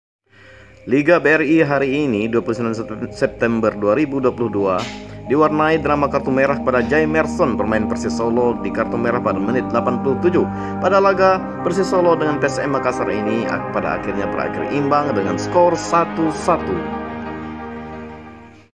PERSIS Vs PSM; 1-1
Liga BRI hari ini,29/9/2022 mempertemukan, Persis Solo Vs PSM Makasar, dengan hasil imbang; 1-1.
Diwarnai drama kartu merah untuk
pemain Persis Jaimerson.